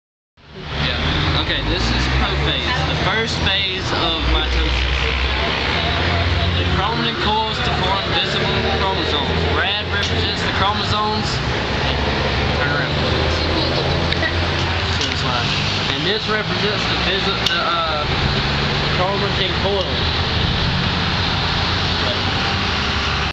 The Stages Of Cell Cycle (Jackass)
Alex Brad Rick And Kevin Do A Jackass Video Of The Cell Cycle